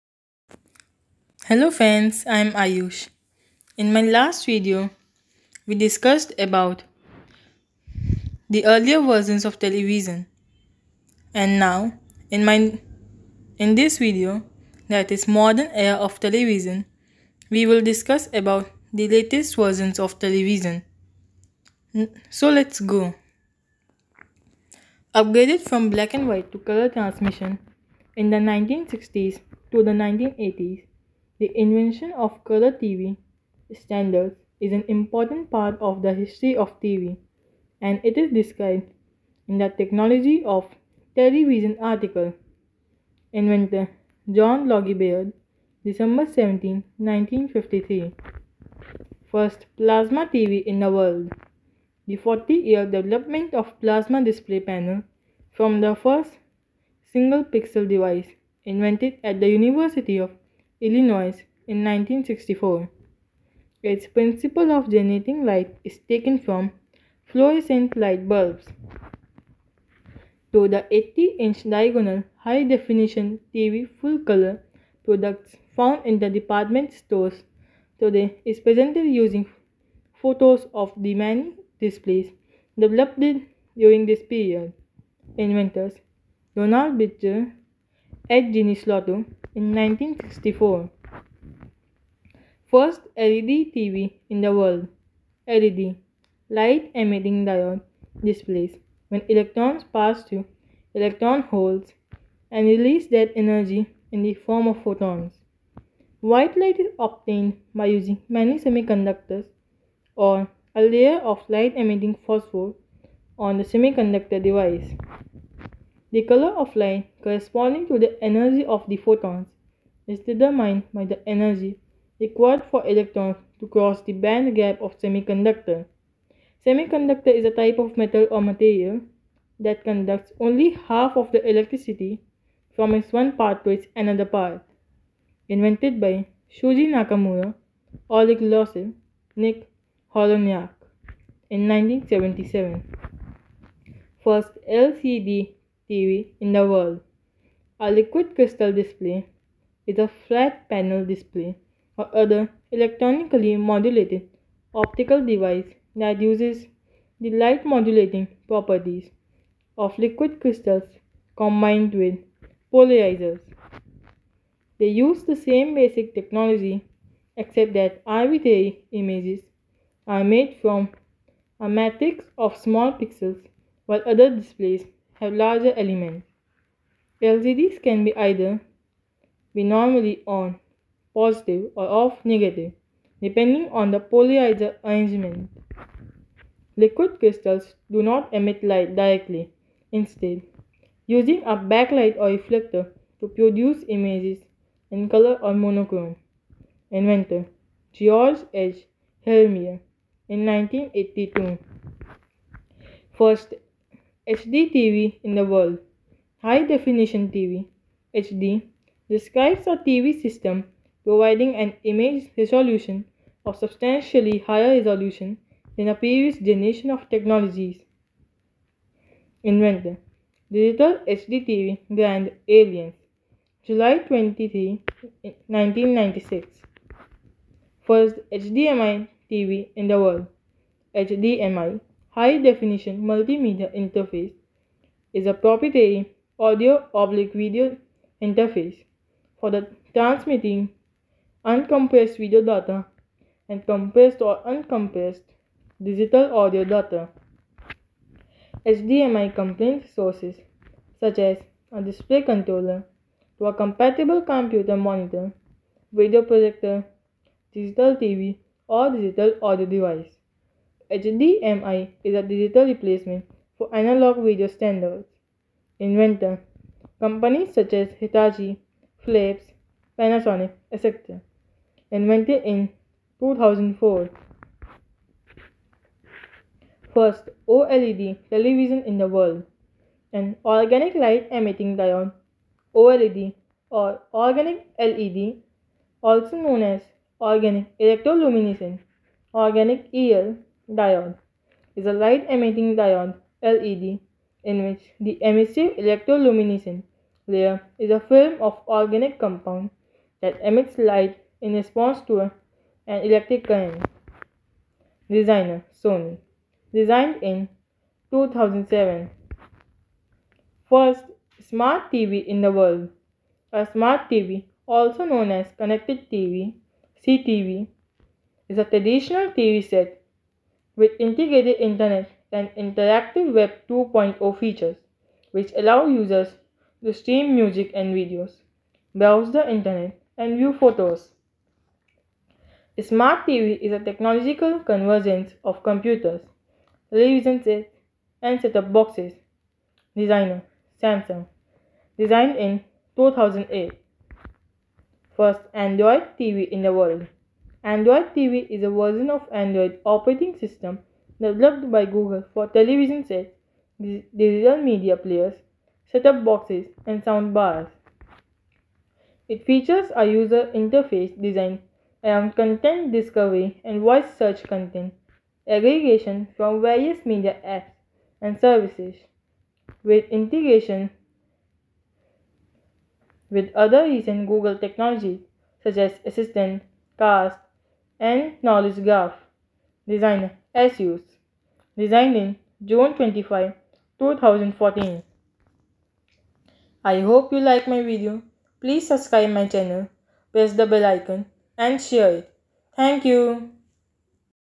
Modern Era of Television
Hello friends, I am Ayush and in this video I have mentioned about latest versions of television. To know more, watch the video. I hope you like it.